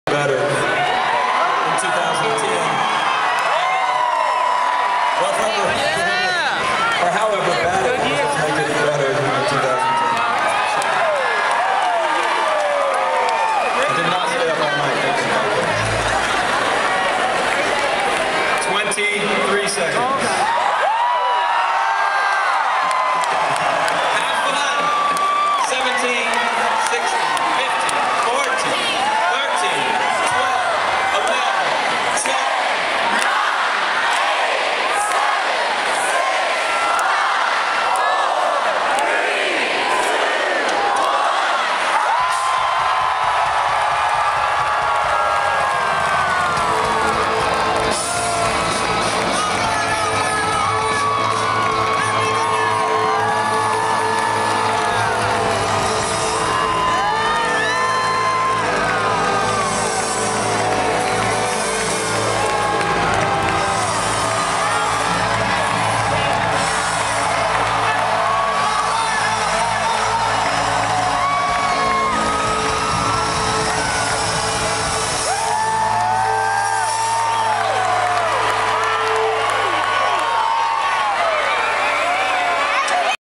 Ringing in the New Year with The Avett Brothers
The Avett Brothers New Year's Eve show in Asheville, N.C.